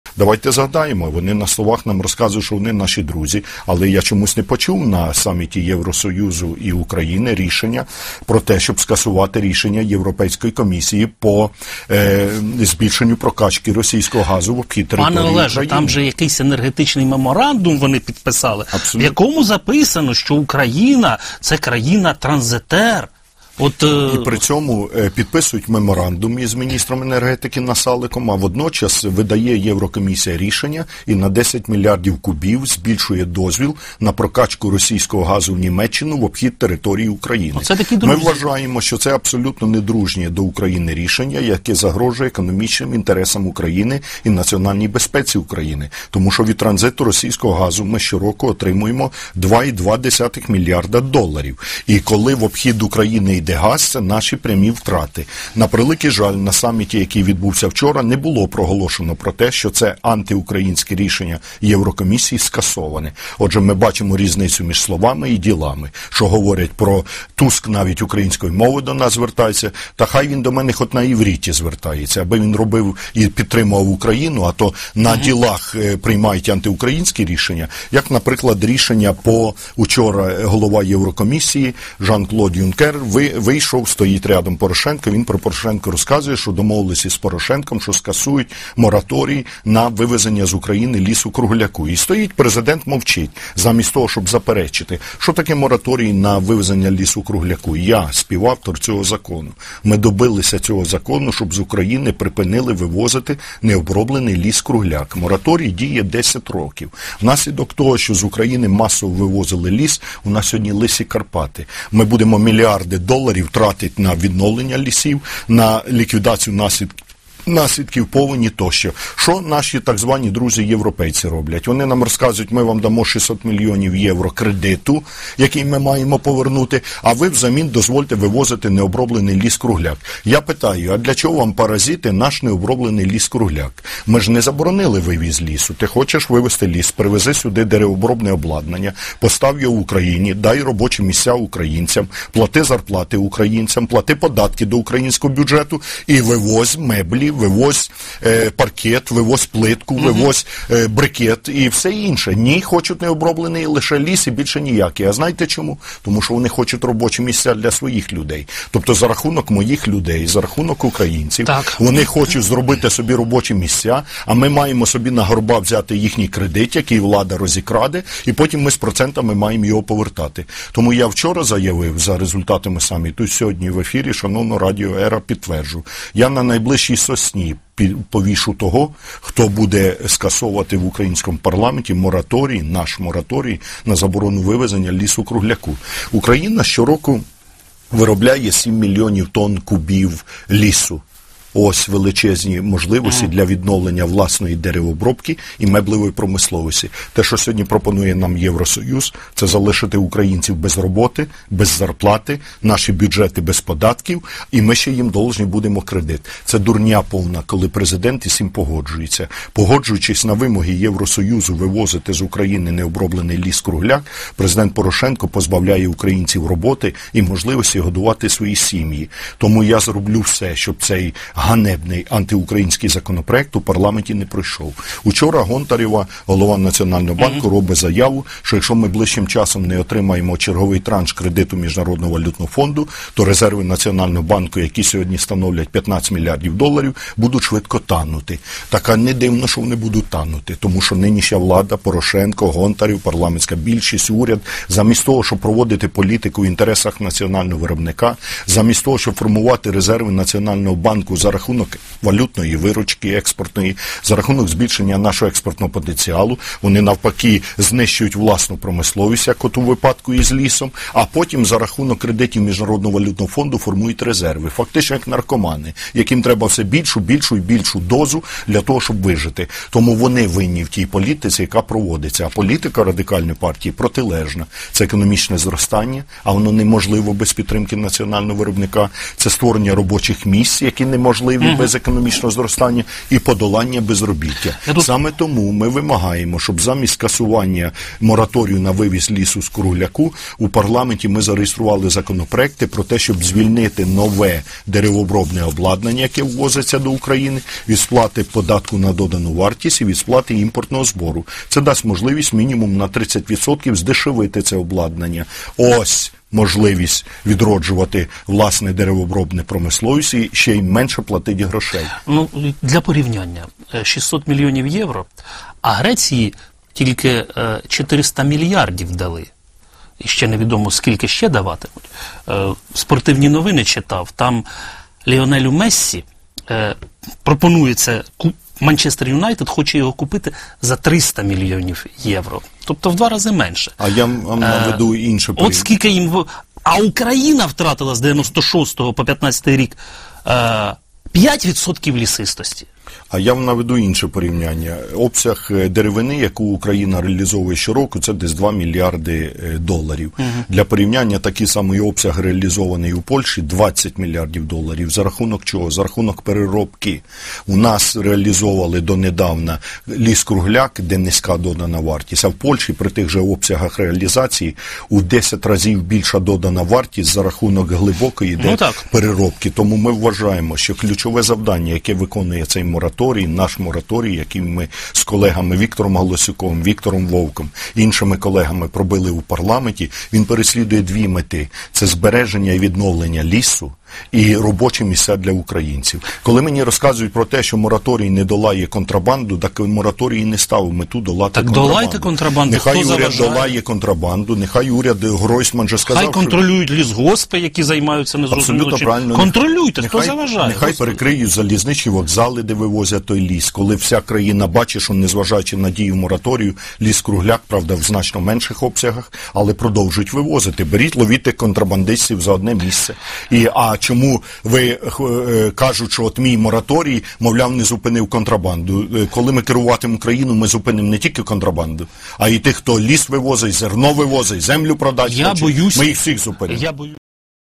Ляшко пропонує повісити на сосні тих, хто хоче продавати ліс
Ті рішення, що пропонує Україні Євросоюз, залишать українців без роботи, зарплати та ще із багатомільйонними кредитами. Про це заявив лідер фракції Радикальної партії Олег Ляшко, підсумовуючи роботу 18-го саміту Україна – ЄС, що відбувся днями у Брюсселі.
«Нам розказують, що вони наші друзі, але я не почув на саміті Україна - ЄС, щоб вони скасували рішення Єврокомісії про збільшення прокачки російського газу в обхід території України. Ми вважаємо, що це абсолютно недружнє до України рішення, яке загрожує економічним інтересам і національній безпеці України. Адже від транзиту російського газу ми щороку отримуємо 2,2 млрд доларів. І на саміті не було жодного слова, що це антиукраїнське рішення. Тож ми бачимо різницю між словами і ділами», - наголосив Ляшко.
Ще більше політика обурила заява голови Єврокомісії Жана-Клода Юнкера про надання Україні 600 мільйонів євро кредиту в обмін на скасування мораторію на вивіз із України необробленої деревини. Нагадаємо, саме фракція Радикальної партії добилася аби Верховна Рада заборонила вивозити із України ліс-кругляк на 10 років.
«Так от, я питаю: для чого вам, паразіти, наш необроблений ліс-кругляк? Ми ж не заборонили вивіз лісу. Хочеш вивозити ліс -  привези в Україну деревообробне обладнання, постав його, дай робочі місця, плати зарплати українцям, плати податки до українського бюджету і вивозь меблі, паркет, плитку, брикети та інше. Ні, вони хочуть лише ліс необроблений, тому що хочуть створити робочі місця для своїх громадян. Я на найближчій сосні повішу того, хто буде скасовувати в українському парламенті наш мораторій», - заявив нардеп.
Не залишила байдужим політика і позиція Президента України в цьому питанні. Олег Ляшко заявляє, що вивозити ліс-кругляк із України неприпустимо. Нардеп нагадав, що внаслідок експорту цільної деревини перлина України - Карпати вже майже лисі і знадобиться не один мільярд гривень на їх відновлення. Лідер фракції Радикальної партії впевнений, замість того, щоб вивозити сировину, потрібно відновлювати власну деревообробну галузь.
«Ми вимагаємо, щоб замість скасування мораторію на вивіз із України лісу-кругляка в парламенті прийняли законопроект, який зареєструвала Радикальна партія. Ми вимагаємо звільнити нове деревообробне обладнання, яке ввозиться в Україну, від сплати податку на додану вартість та імпортного збору. Це дасть можливість мінімум на 30% здешевити це обладнання. Ось можливість відроджувати власну деревообробну промисловість та ще й менше платити грошей», - впевнений політик.
На останок Олег Ляшко додав, що мораторій на вивезення лісу-кругляка із України – це не тільки можливість збереження лісу, але і нові робочі місця. Тож фракція Радикальної партії і надалі захищатиме українців.